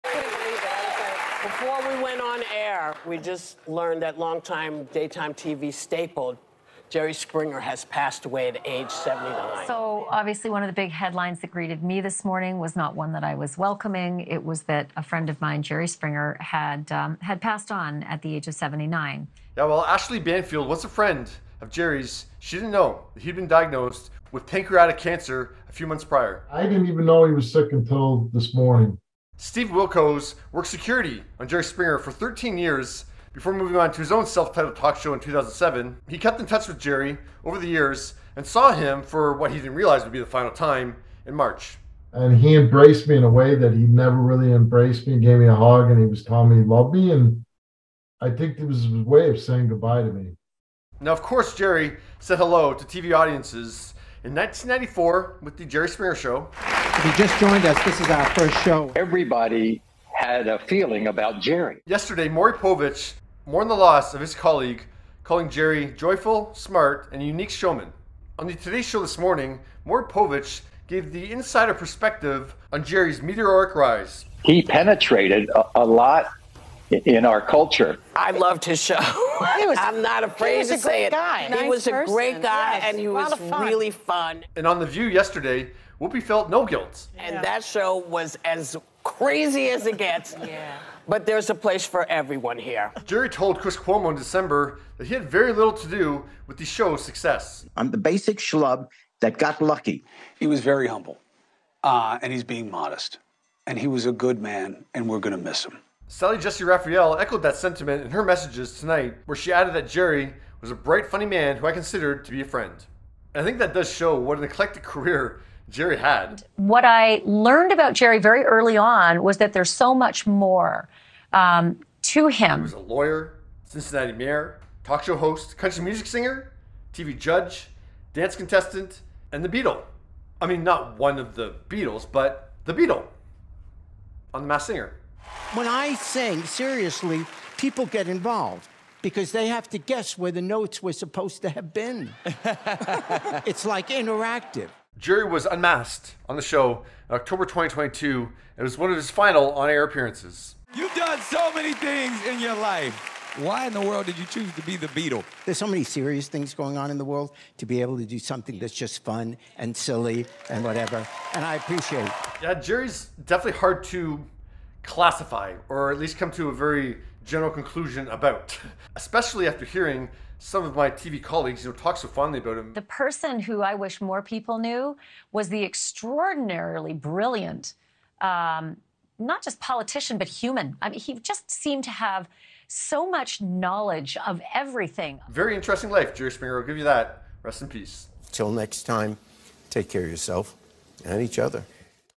TV Hosts React To Jerry Springer's Death
Graeme O'Neil takes a look at the reaction of other TV hosts after Jerry Springer passed away at the age of 79.